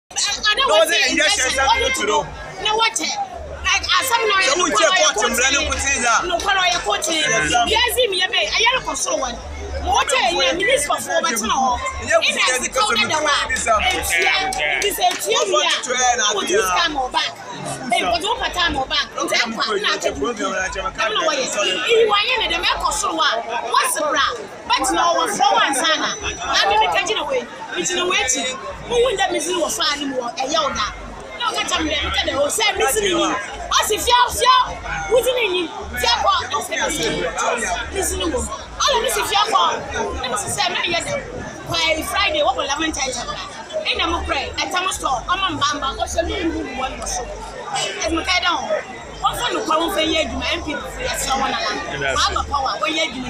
Woman Narrates Painful Experience Before Getting A Store At the New Kotokuraba Market
Woman Narrates the difficulties Before Getting A Store At the New Kotokuraba Market